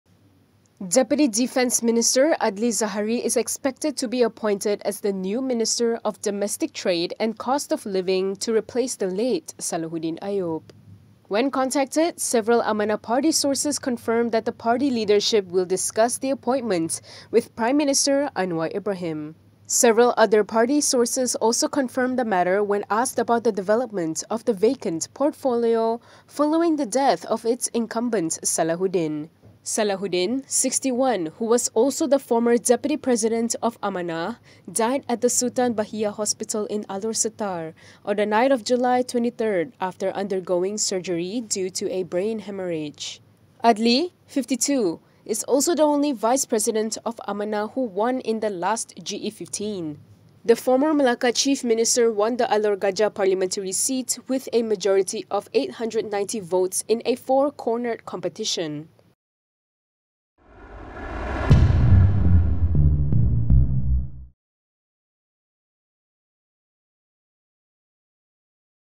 Amanah veep Adly to be made domestic trade minister - sources
Deputy Defence Minister Adly Zahari is expected to be appointed the domestic trade and cost of living minister, to replace the late Salahuddin Ayub.

When contacted, sources from Amanah confirmed this with Malaysiakini.